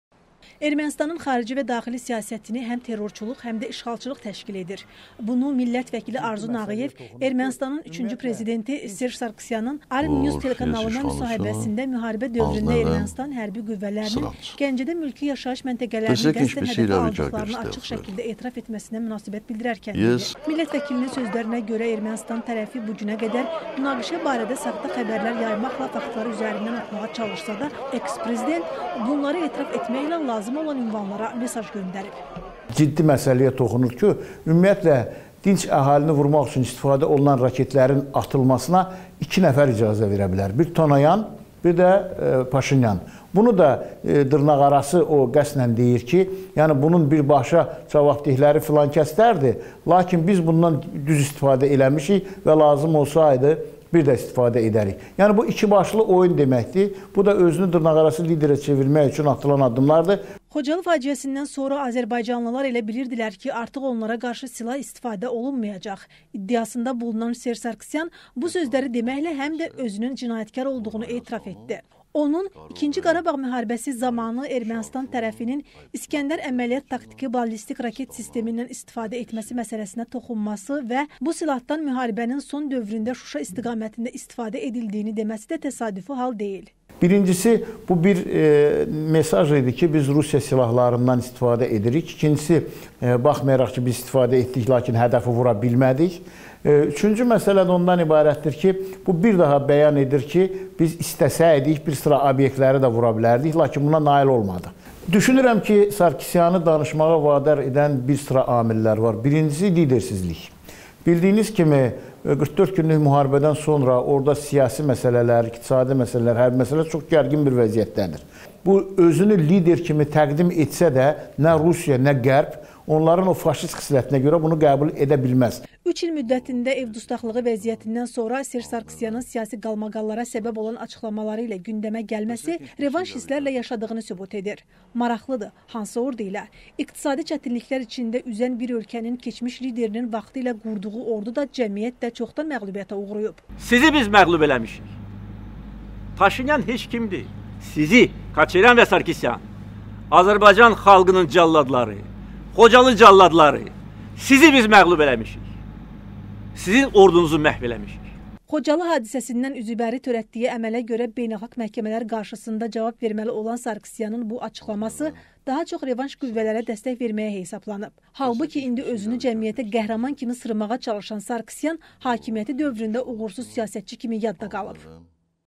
“Qəhrəman“ cildində cinayətkar
S.Sarqsyan revanş hisslərlə yaşadığını sübut edir